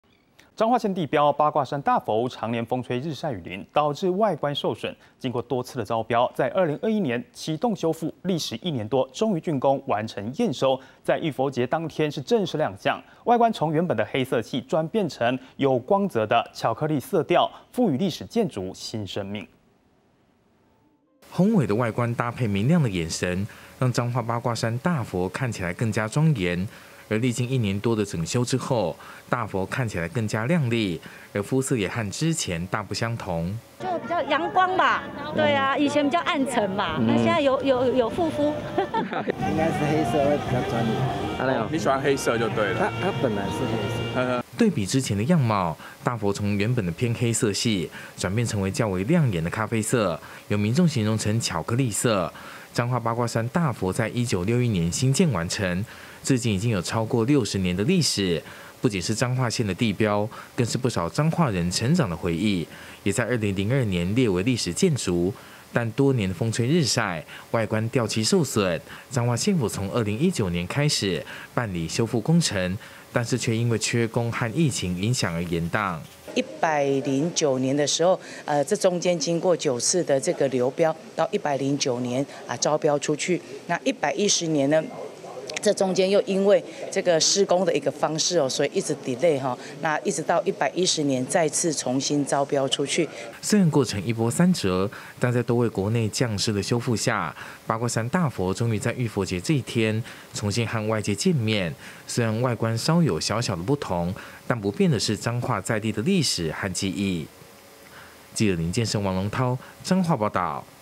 八卦山大佛整修完工 浴佛節正式對外亮相｜20230527 公視晚間新聞
彰化縣地標八卦山大佛，長年風吹日曬雨淋，導致外觀受損，經過多次招標，在2021年啟動修復，歷時一年多終於竣工，完成驗收，在浴佛節正式亮相。外觀也從原本黑色系，轉變成有光澤感的巧克力色調，賦予歷史建築新生命。